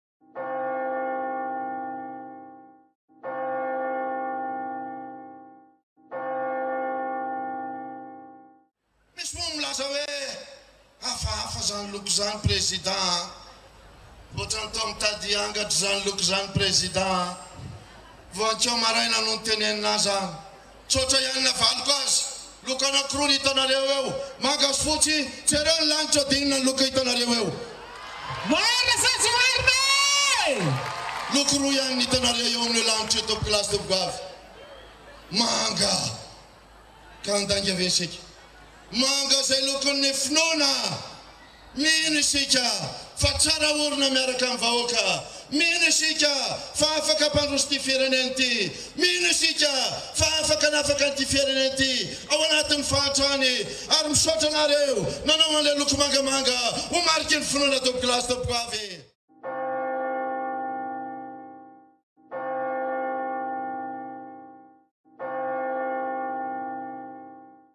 Hery Rajaonarimampianina MANGA ! 10 07 2015
MANGA ! hoy ingahy Hery Rajaonarimampianina misy "ressorts". Ny teo aloha ihany izany no miverina: mino fotsiny ihany... fa manga ny lokon'ny finoana !
Tena sao dia ianao Andriamatoa no ho mangana atsy ho atsy ?